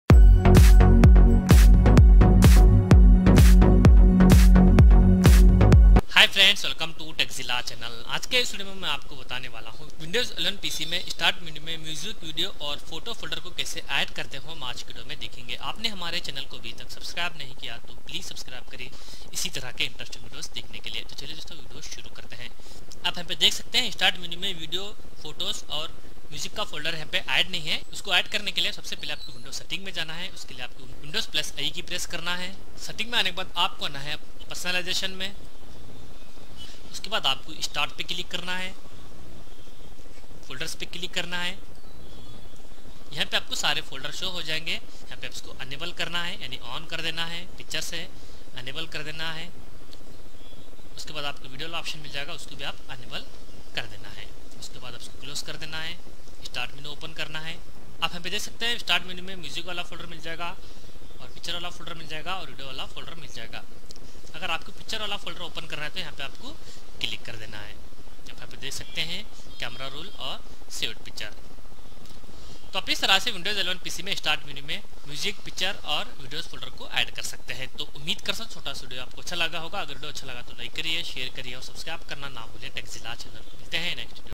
How to Add Music, Video and Photo Folder In Start Menu On Windows 11
Folders Add in Start menu in windows 11
Photo music and video folder add in start menu in windows